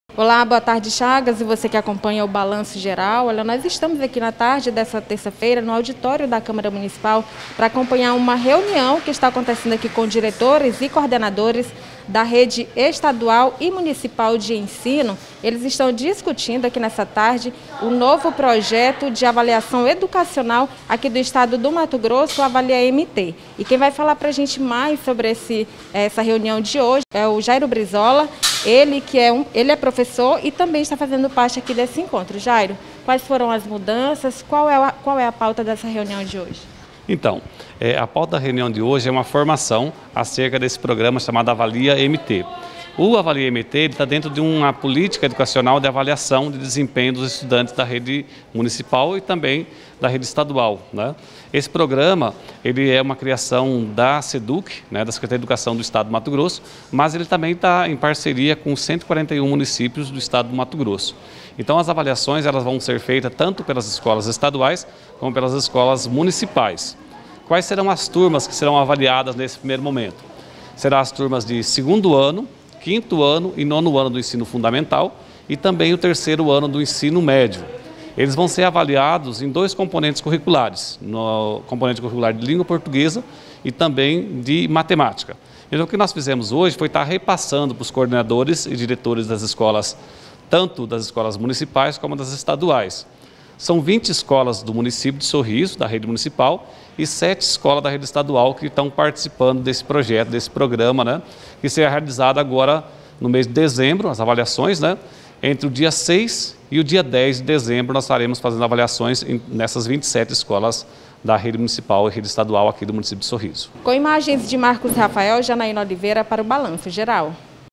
25/11/21 Diretores e coordenadores se reúnem para discutir a política de avaliação Educacional
Diretores e coordenadores participam de reunião para discutir a política de avaliação Educacional em Sorriso